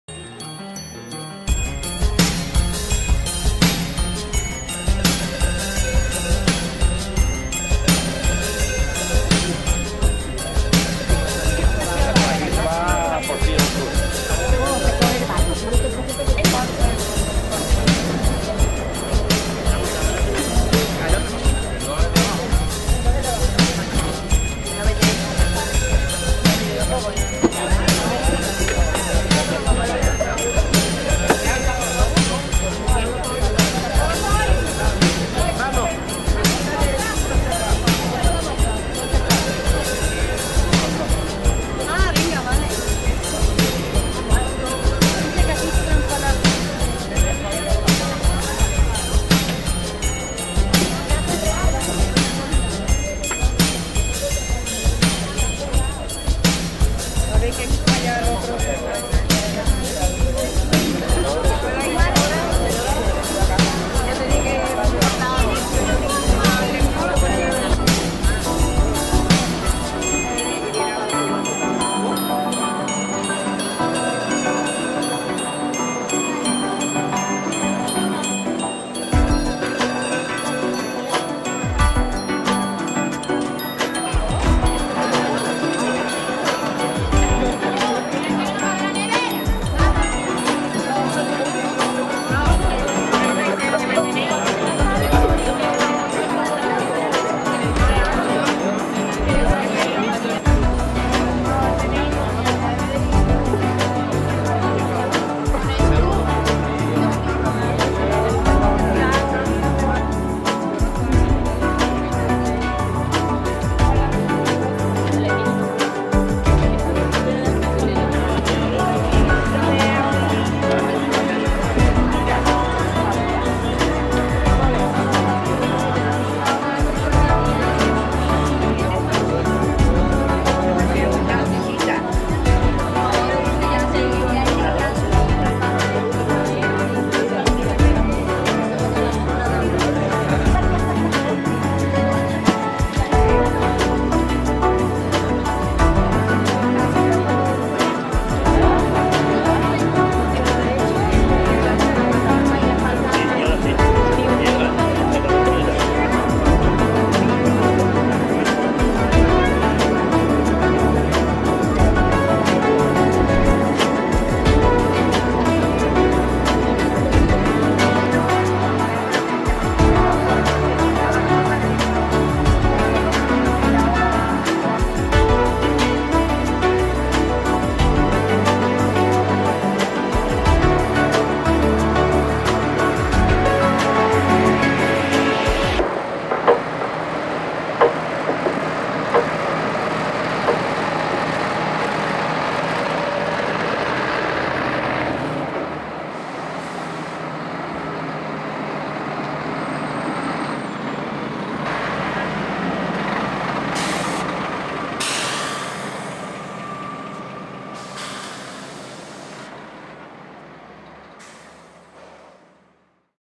Los jueves al sol. Desde Gines a Mazagón
Los jueves al sol.
Salida en autobús desde Gines a la playa de Mazagón.
Organiza: Ayuntamiento de Gines.